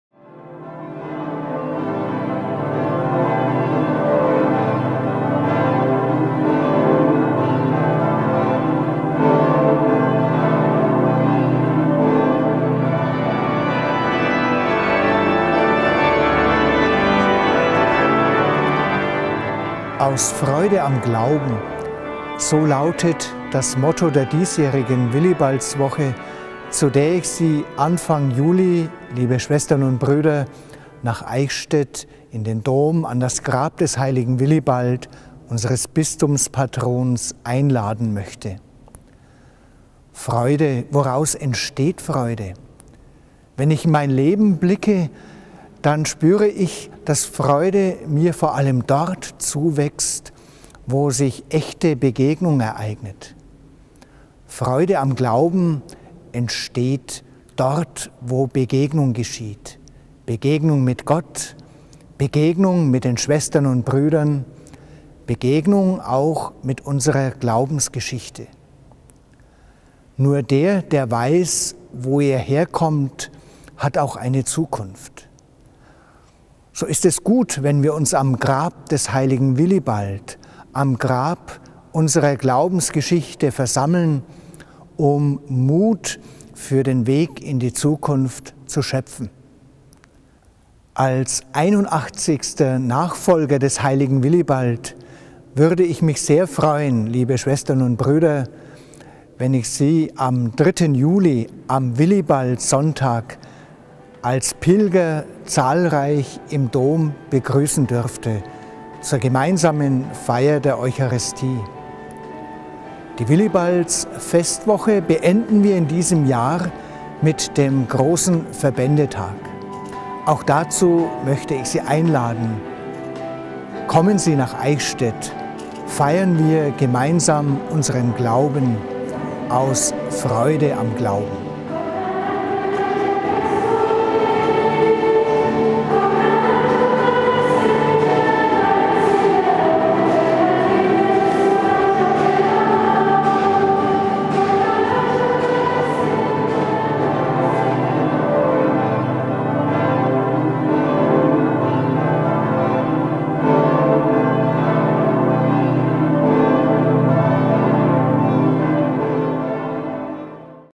Willibaldswoche: Aus Freude am Glauben
Die Willibaldswoche 2011 findet vom Samstag, 2. Juli, bis Sonntag, 10. Juli, in Eichstätt statt. Sie will durch verschiedene Angebote beitragen, dass sich viele Gläubige aus der Diözese am Grab des Bistumspatrons einfinden und Glaubens-Gemeinschaft erleben. Das Thema der Festwoche lautet: "Aus Freude am Glauben".